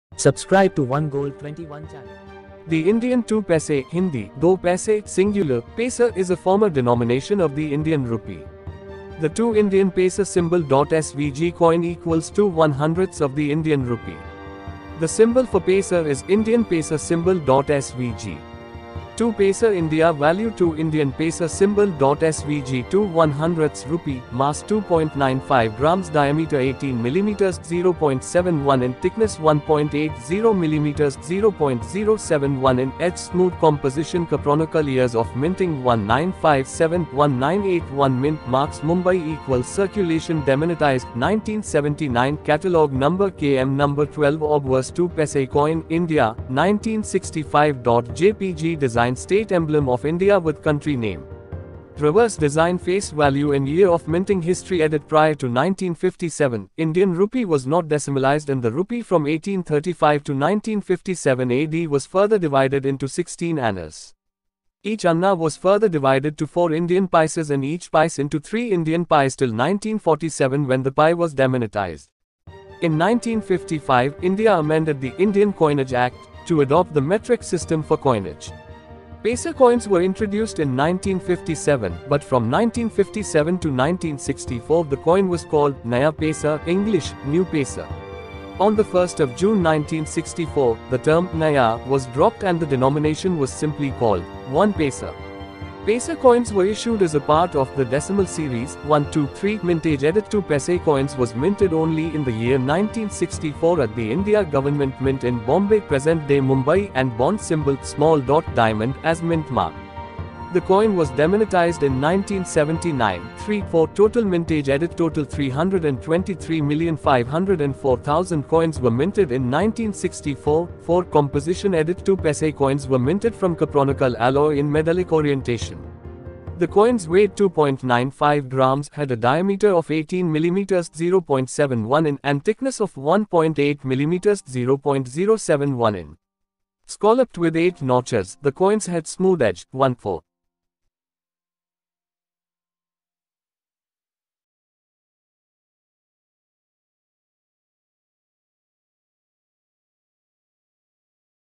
720k file: India 2 paisa coin full review in English
Prior to 1957, Indian rupee was not decimalised and the rupee from 1835 to 1957 AD was further divided into 16 annas. Each anna was further divided to four Indian pices and each pice into three Indian pies till 1947 when the pie was demonetized. In 1955, India amended the "Indian Coinage Act" to adopt the metric system for coinage. Paisa coins were introduced in 1957, but from 1957 to 1964 the coin was called "Naya Paisa" (English: New Paisa). On 1 June 1964, the term "Naya" was dropped and the denomination was simply called "One paisa". Paisa coins were issued as a part of "The Decimal Series".[